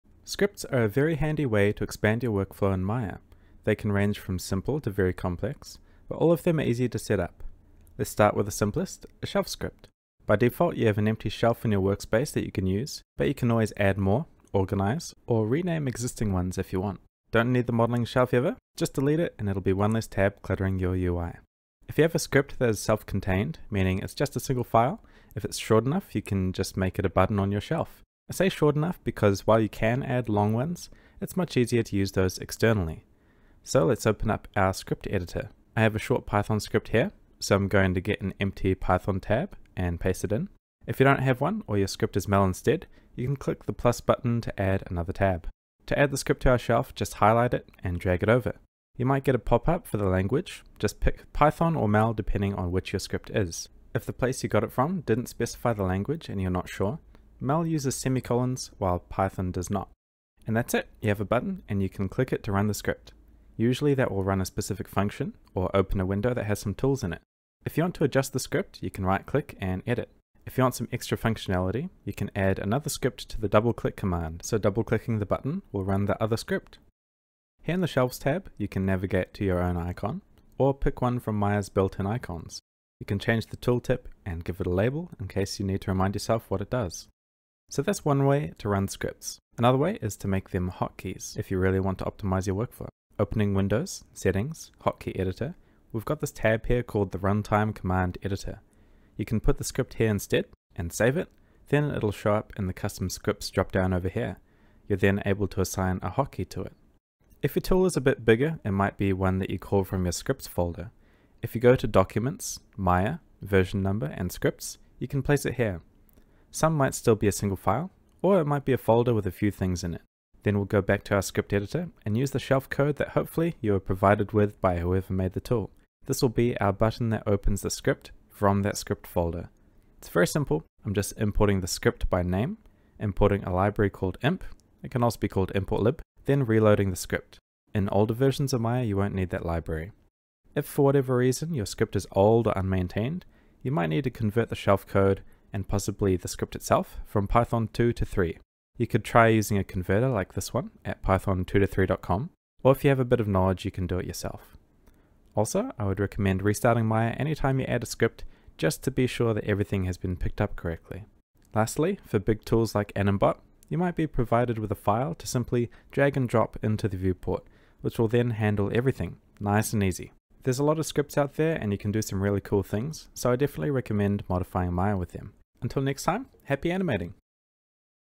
How to Add Scripts to Maya
Adding scripts to Maya is a great way to improve your workflow!
Please let us know what you'd like other tutorials for.

Timestamps:
Intro 0:00
Shelf script 0:21
Edit shelf button 1:07
Hotkeys 1:29
External script 1:47
Python2to3 2:20
Drag n Drop 2:41